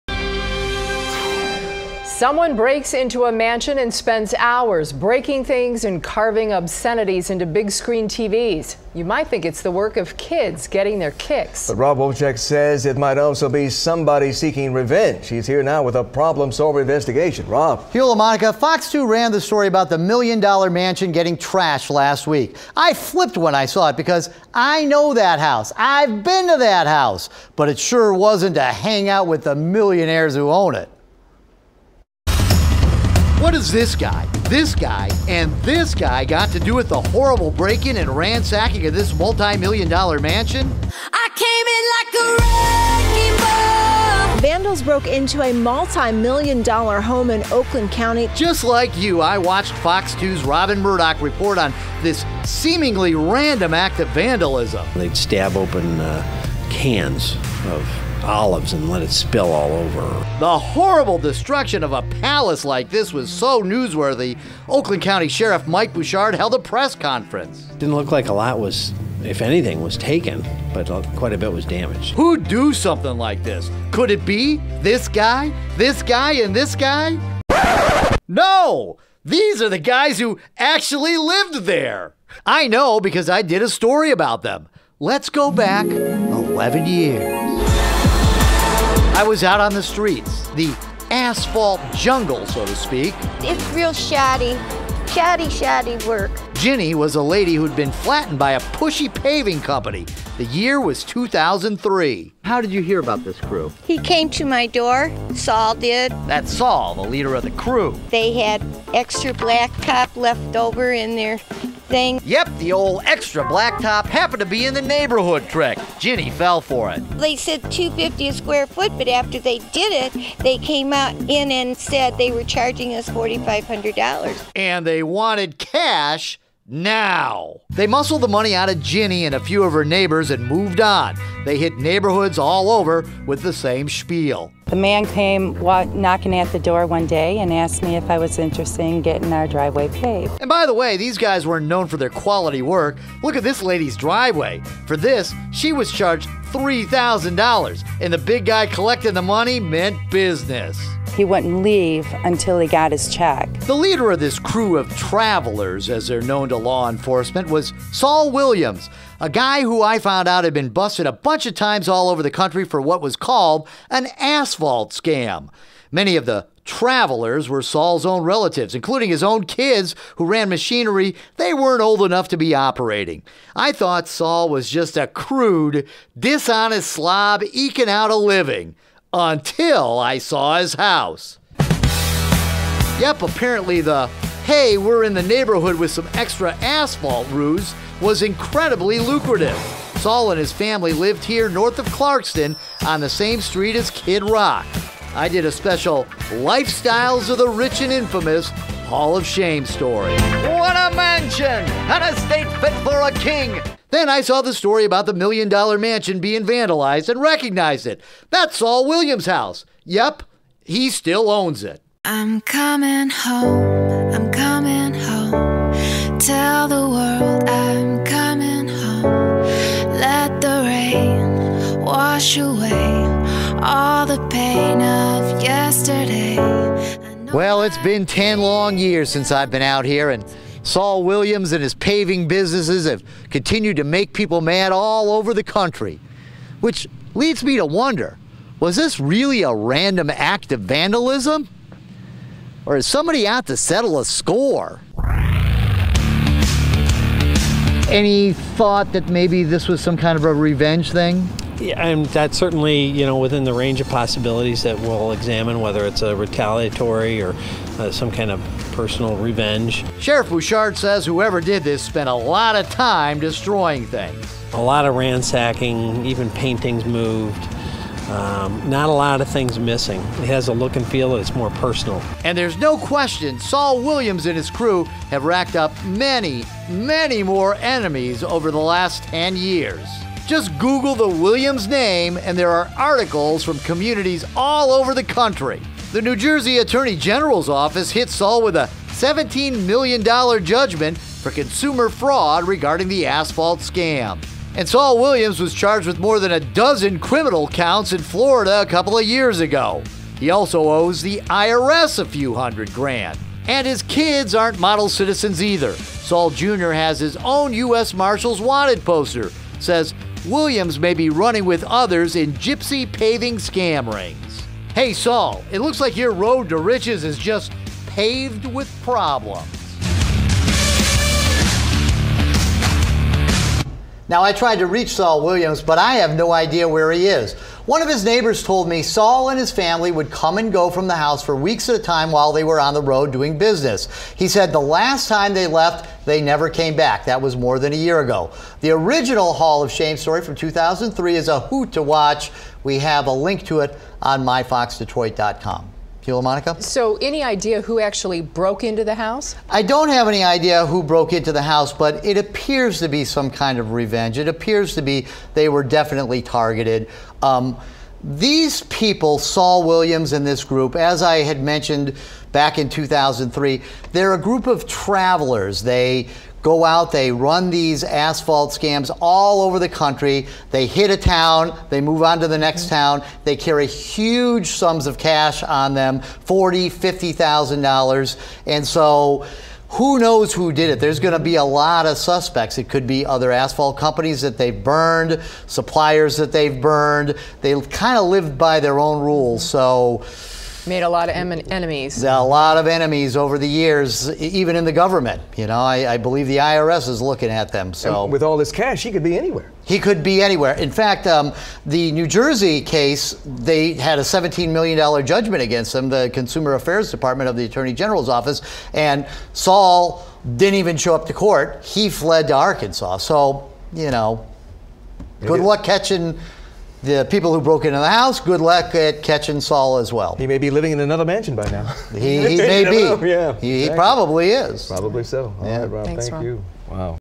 Wolchek Investigation: Random act of vandalism or revenge?
Fox 2's Rob Wolchek recognized a vandalized million dollar house in the news recently - because he's been there before.
What does Saul Williams have to do with the horrible break in and ransacking of this million dollar mansion?